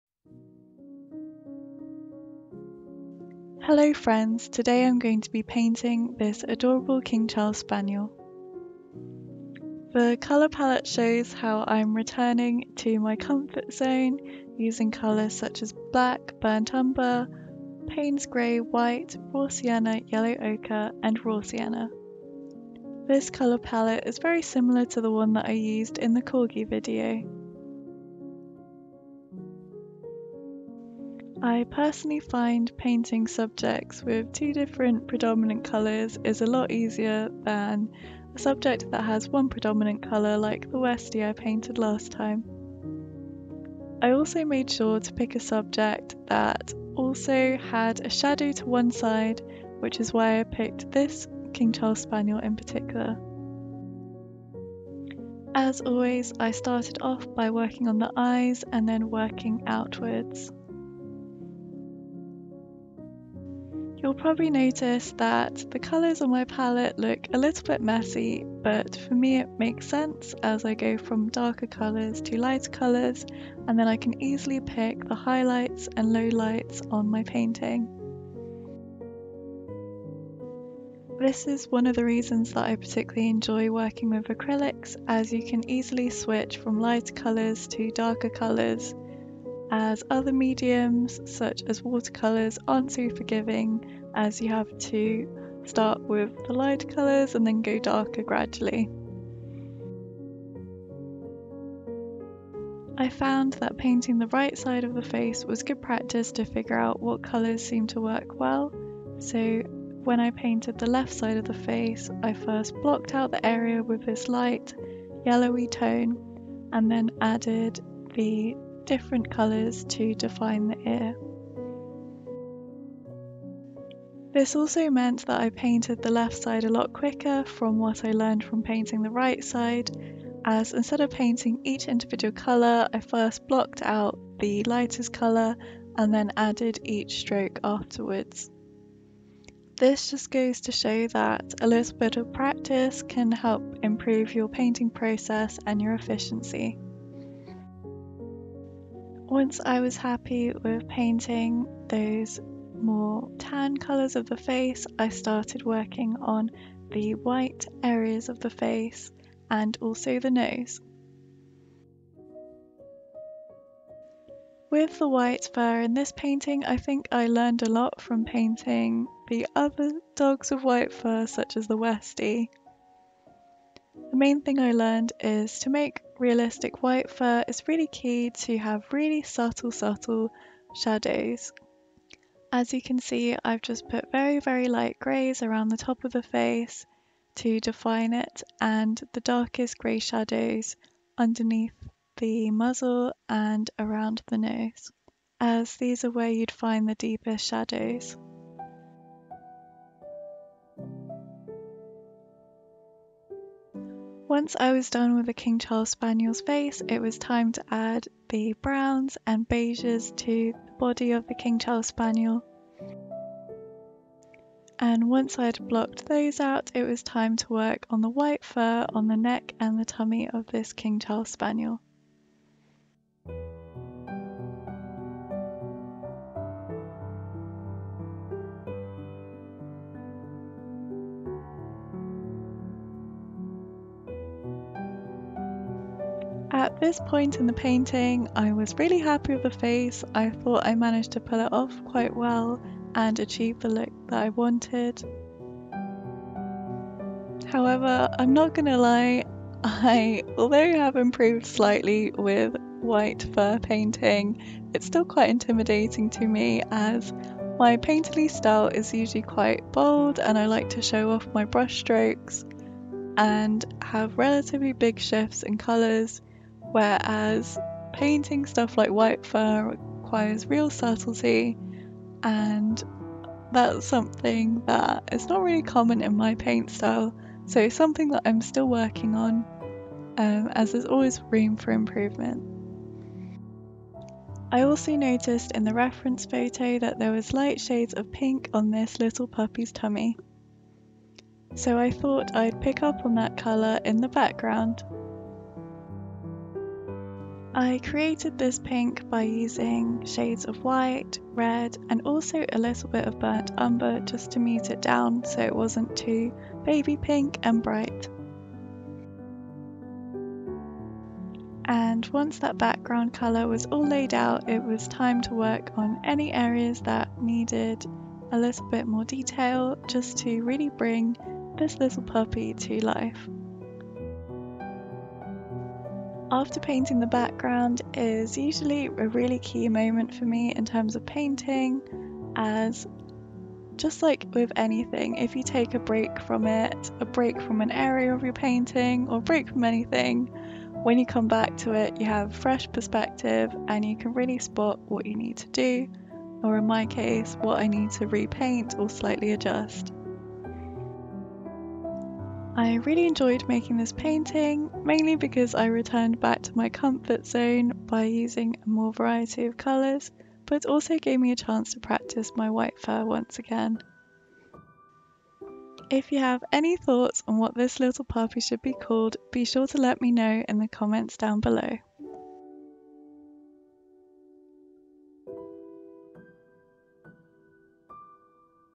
King Charles Spaniel in Acrylic │ Speed Paint
Here is my time-lapse painting of the little King Charles Spaniel puppy in acrylic. I mention a challenges about painting white fur in this one, how I streamline my painting process and I also talk about my thoughts throughout the painting. Hope you like it as I really enjoyed painting this dog for my little puppy series!

MATERIALS
Primer - Boldmere White Gesso
Paper - Daler Rowney Mixed Media Spiral Pad A5
Brushes: KINBOM 9 Pieces Fine Detail Paint Brush Minature Painting Brushes Kit Mini Paints Brush
Set for Acrylic
Paints - Pebeo Studio Acrylic Paint in
Mars Black
Burnt Umber
Naples Yellow
Titanium White
Yellow Ochre